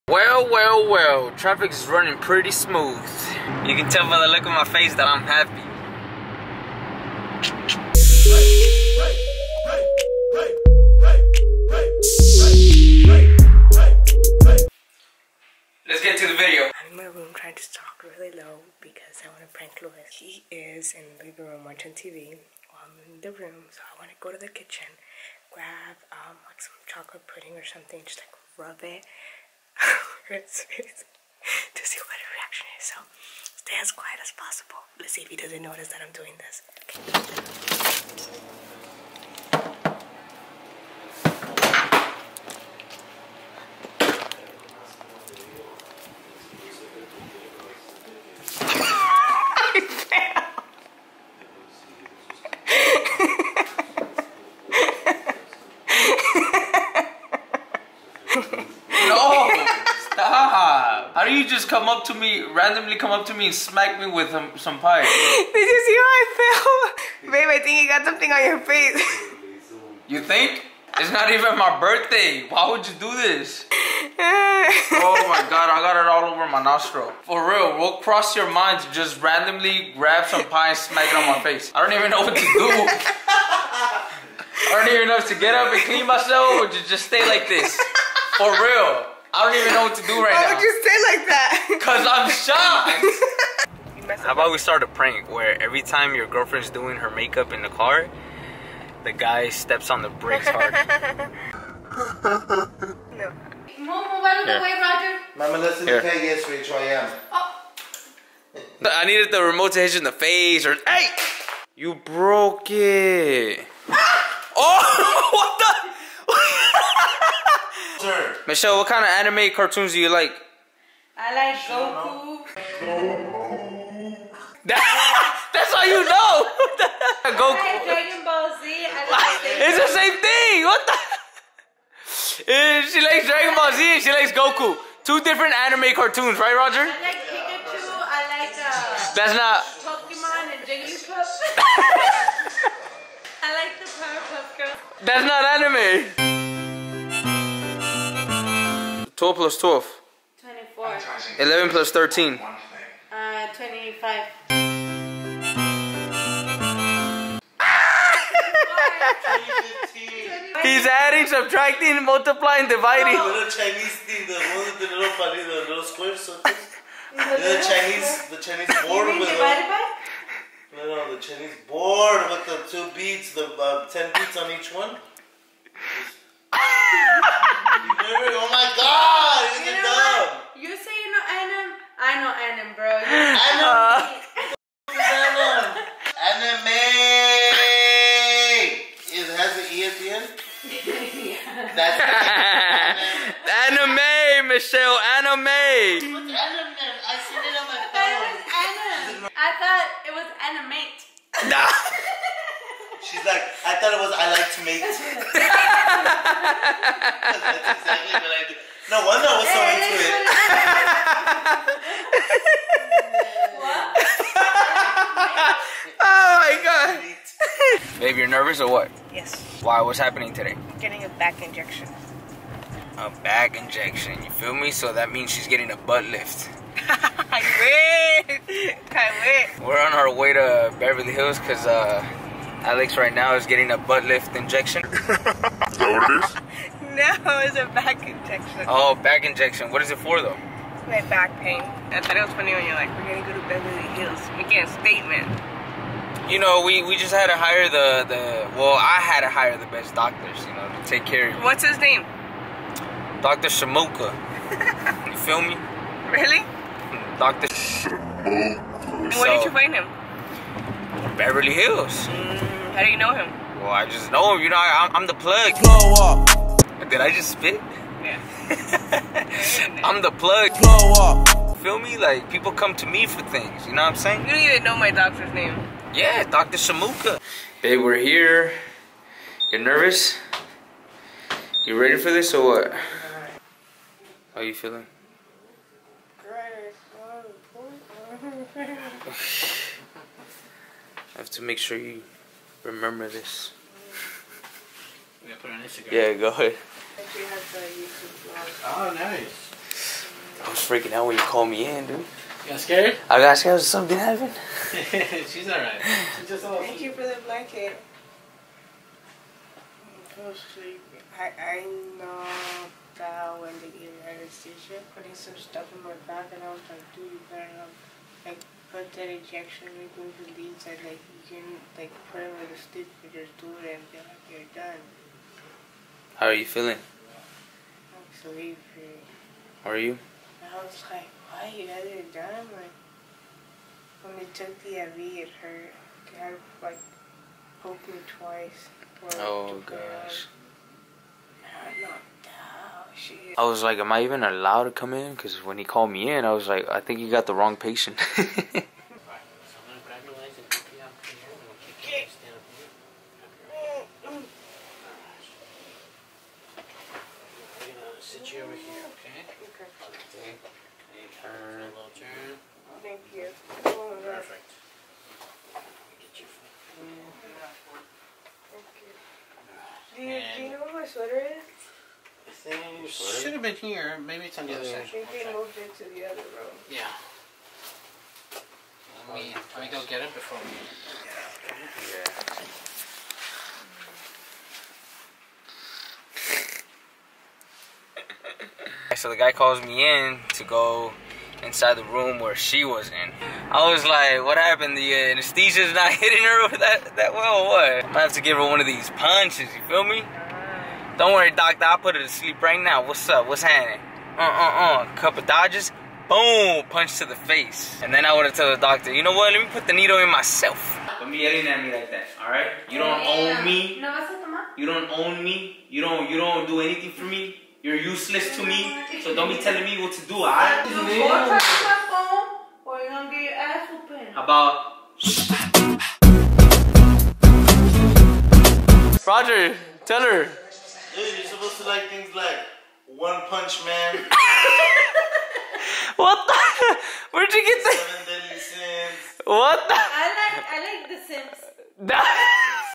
HOW TO GET YOUR BOYFRIEND OFF HIS PHONE! | Luis Angel
My angry girlfriend slaps me while I was distracted! watch my reaction!
The Vlog continues here!👇🏼

cheezapp @ Los Angeles